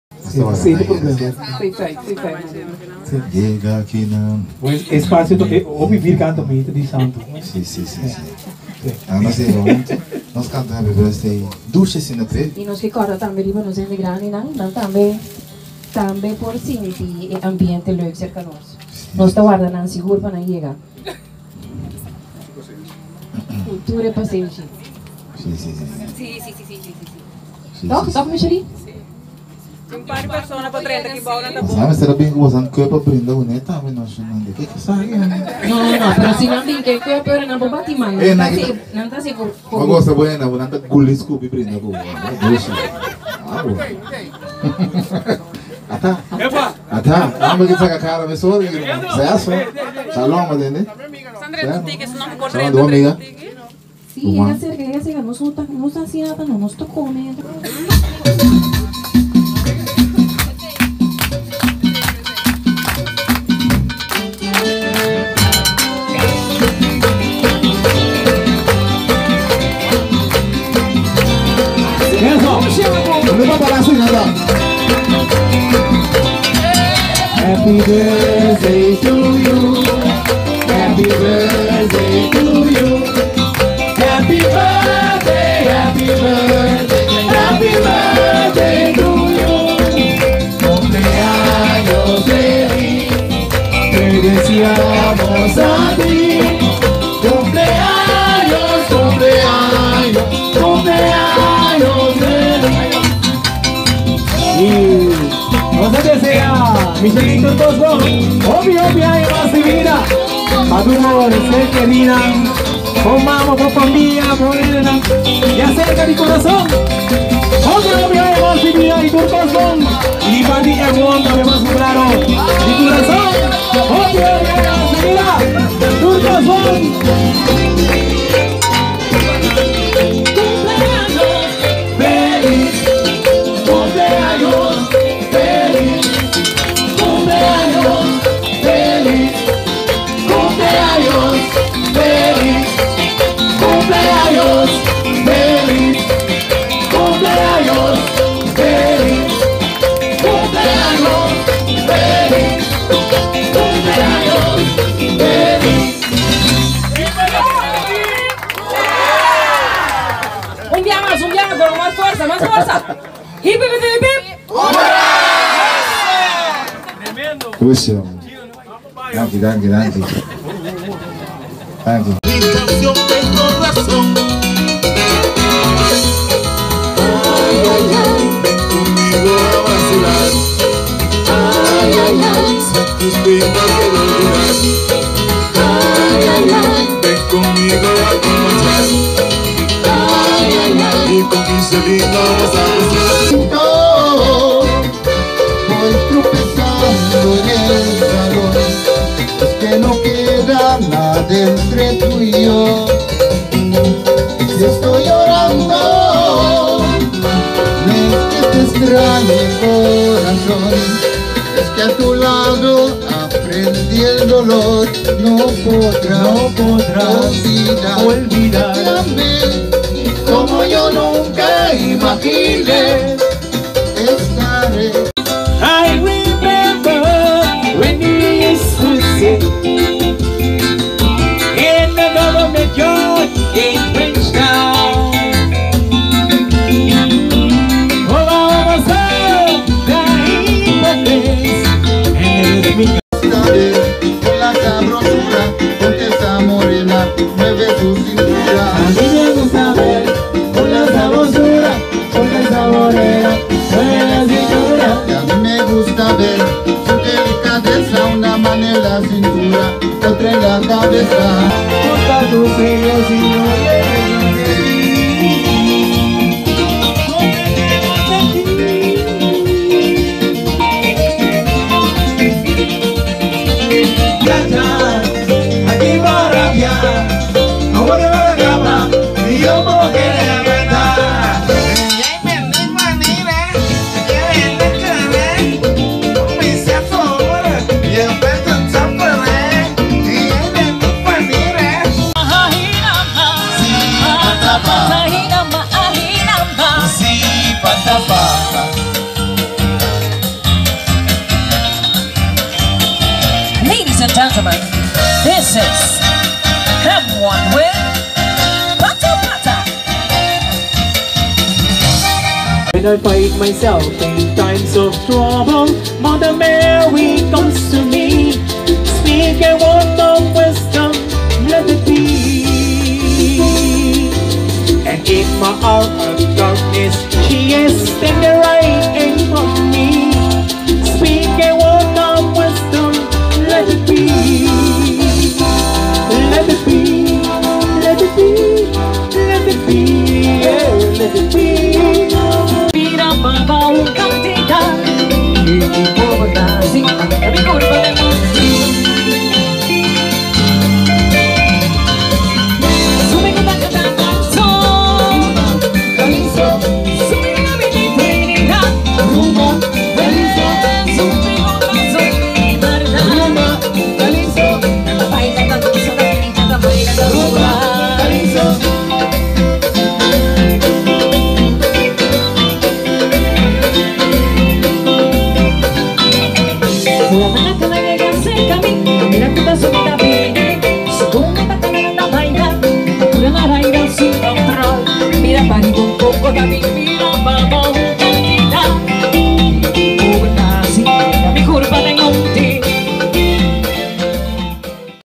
M-one AT beach Party - Curacao Video Bands
M- one partied with high level music to these people today , and here is a small summary